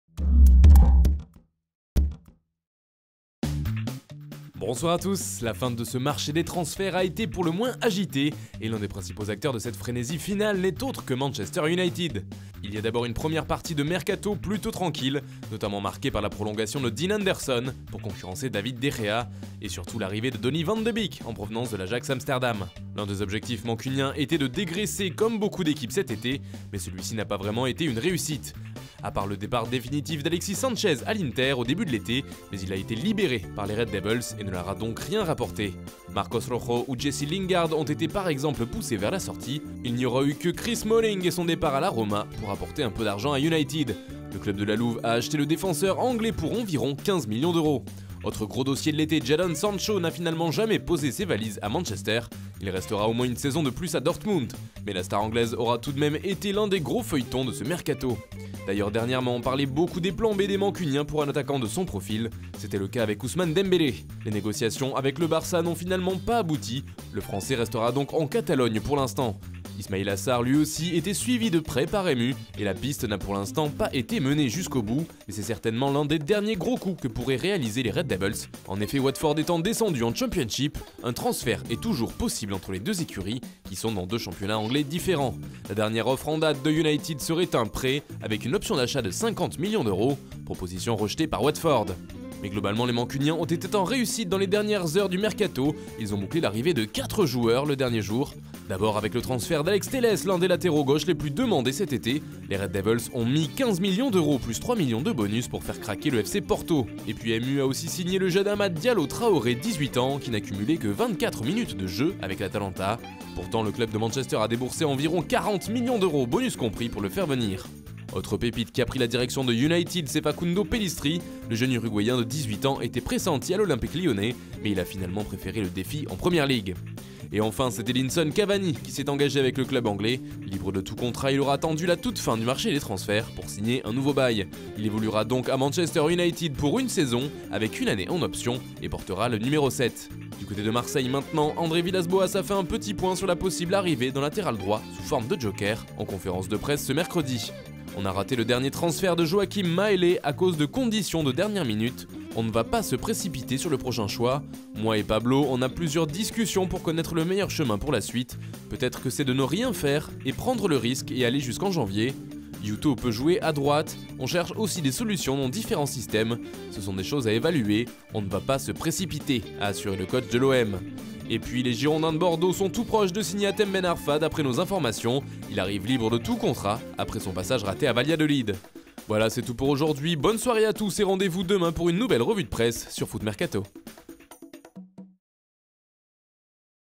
La dernière ligne droite de folie de Manchester United | Journal du Mercato
Alors que le feuilleton Jadon Sancho (20 ans) s'est éternisé lors de ce mercato estival 2020, Manchester United n'a finalement pas réussi à s'offrir le jeune anglais. Les Red Devils se sont donc consolés lors des derniers jours en enchaînant les signatures.

Du talent, de l'expérience, mais aussi des renforts ciblés, la direction du club s'en sort avec des recrues, mais pas forcément celles qu'elle visait. Retour sur les derniers jours agités du mercato mancunien.